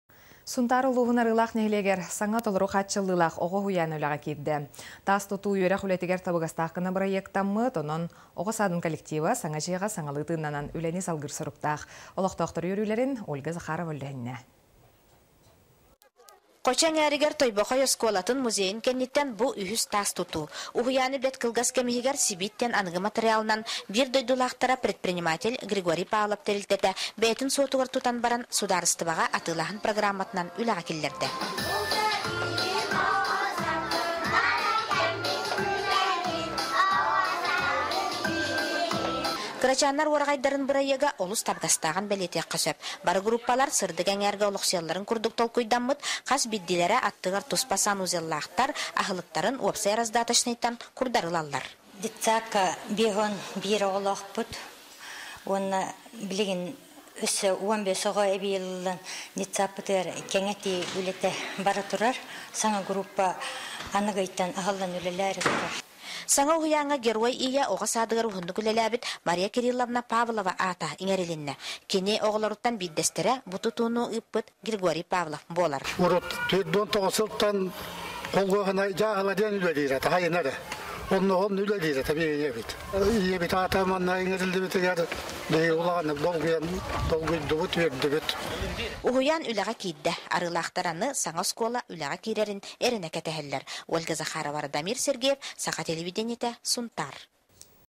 Сунтаар улууһун Арыылаах нэһилиэгэр саҥа оҕо уһуйаана үлэҕэ киирдэ
Таас тутуу үөрэх үлэтигэр табыгастаах гына бырайыактаммыт. Онон оҕо саадын коллектива саҥа дьиэҕэ саҥалыы тыыннанан үлэни салгыыр соруктаах.